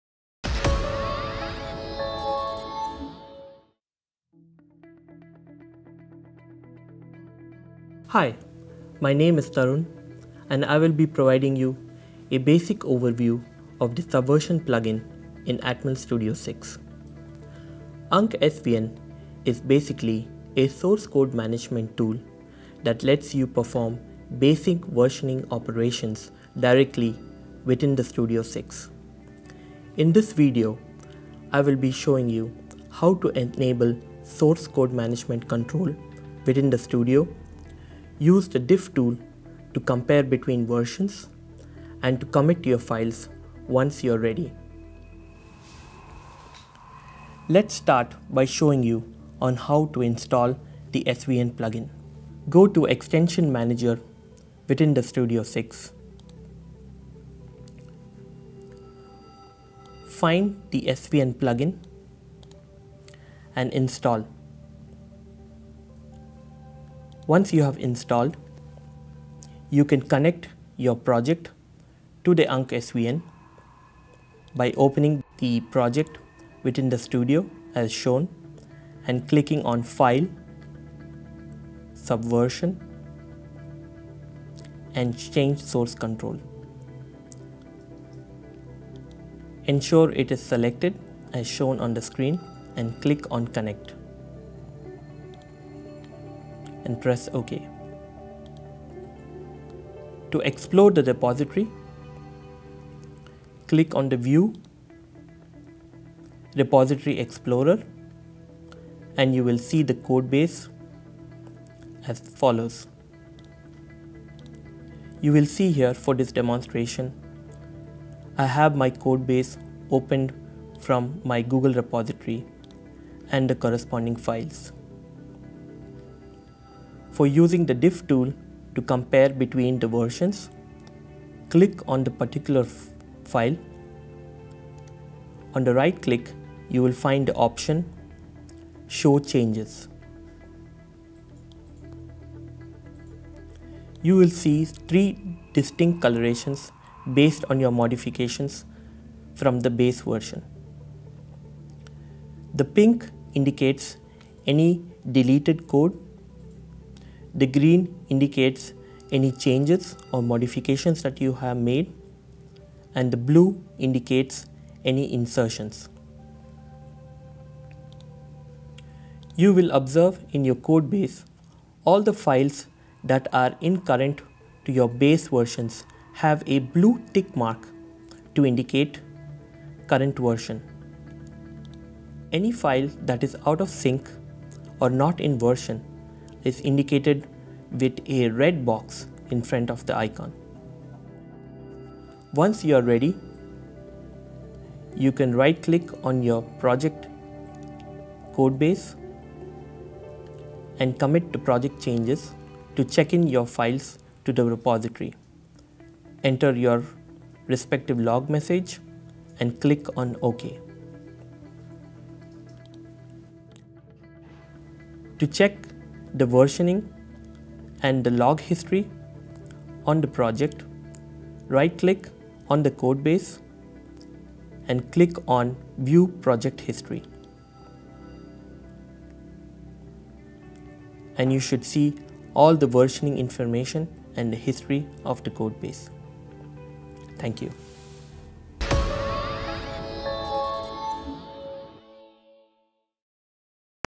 Using the SVN Plug-in for Atmel Studio
The video shows how you can use the SVN Plug-in Atmel Studio 6.0 to perform basic versioning operations within the Studio 6.0. We demonstrate how you make and track changes to the code and finally check in to your repository.